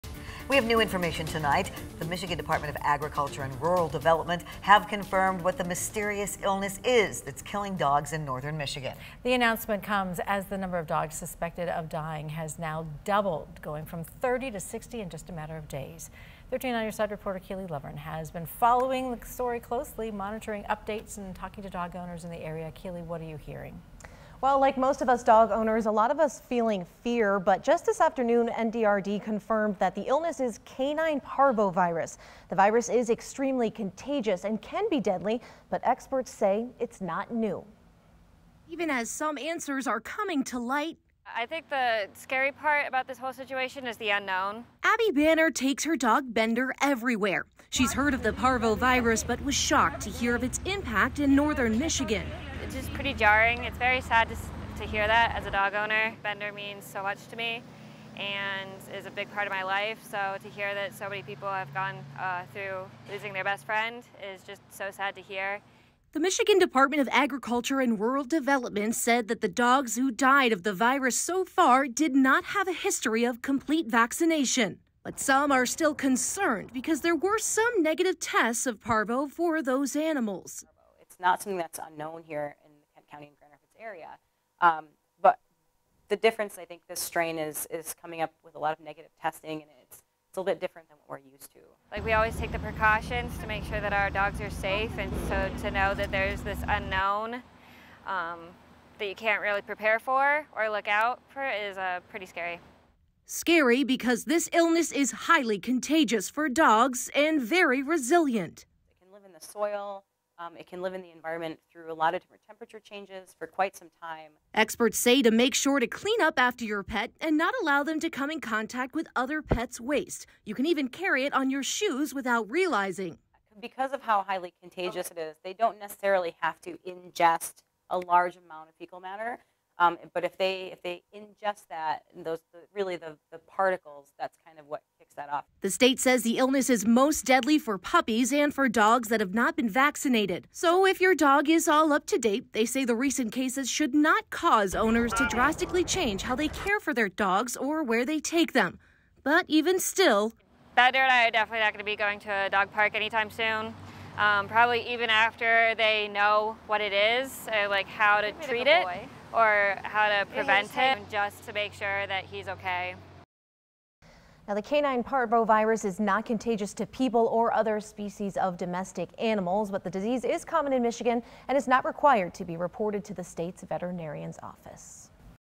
Officials determine illness killing dogs in N. Michigan is parvovirus as death toll doubles
The announcement comes as the number of dogs suspected of dying has now doubled, going from 30 to 60 in just a matter of days.